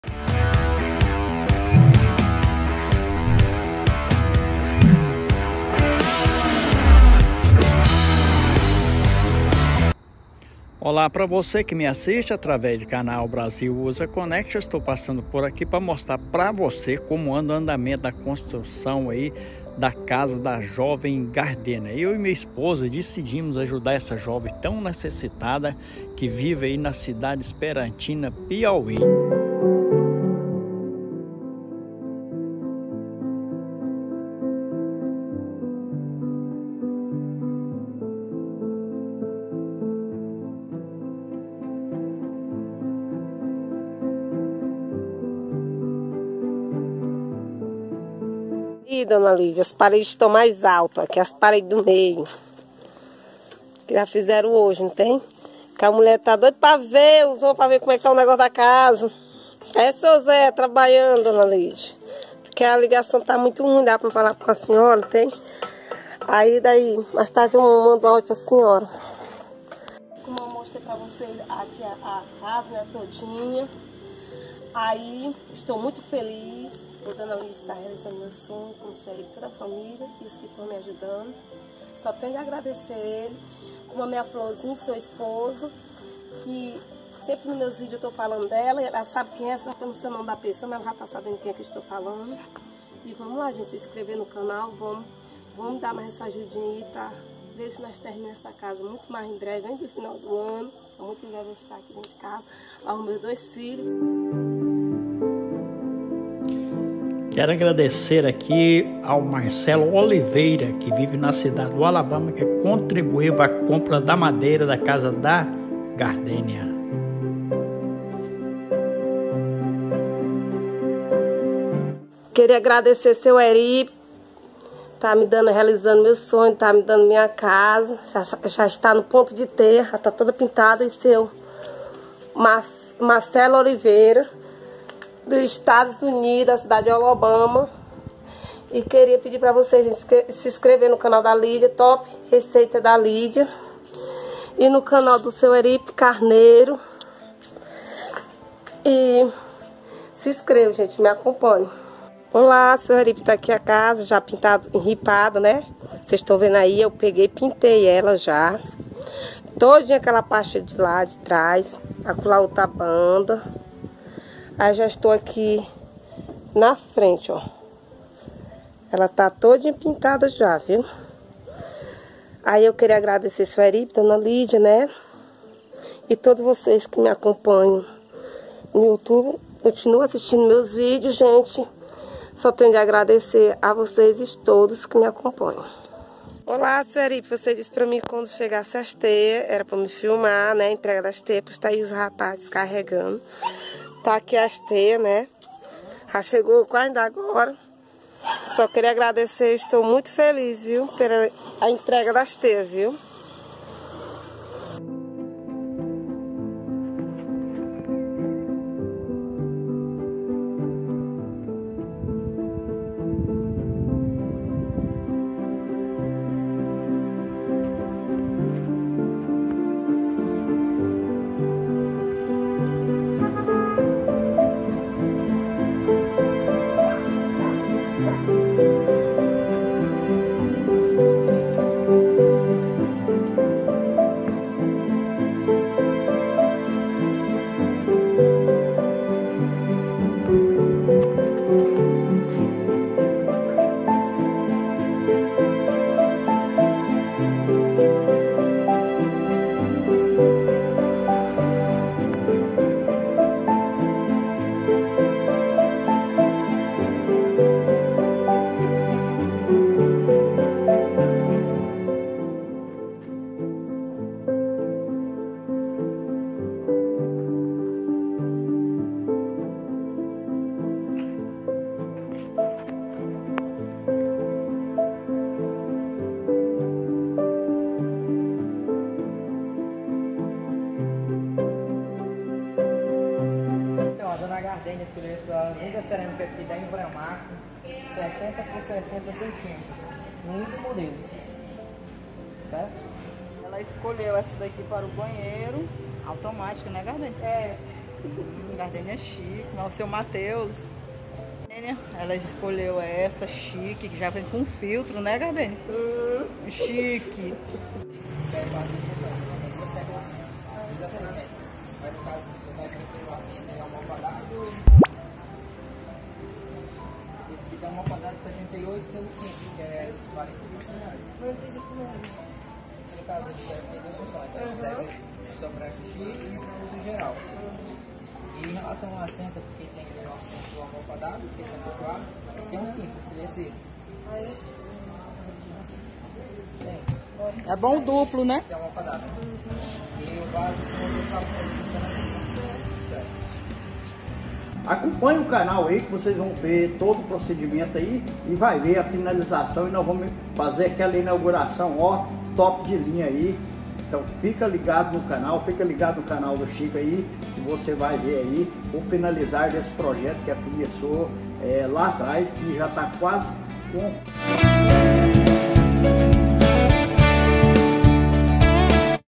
Como realizar um sonho/Piauí
Olá, está  chegando  a reta final da construção da casa da Jovem Gardênia que vive na cidade de ESPERANTINA/Piauí. Um projeto que nasceu no coração de um casal de brasileiros que vive nos Estados Unidos. O casal Euripedes Carneiro e LIDIA Carneiro não estão medindo esforços pra se tornar realidade o sonho da jovem Gardênia de ter  a tão sonhada casa própria. A jovem Gardênia que vive na cidade de ESPERANTINA Piauí Brasil. Ela é  mãe de dois filhos e leva a vida com muita dificuldade. Se alguém quiser contribuir entra em contato pelo telefone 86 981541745. Depois de terminada a casa ela necessita comprar os utensílios de casa e alimentos pros filhos. Que Deus os abençoe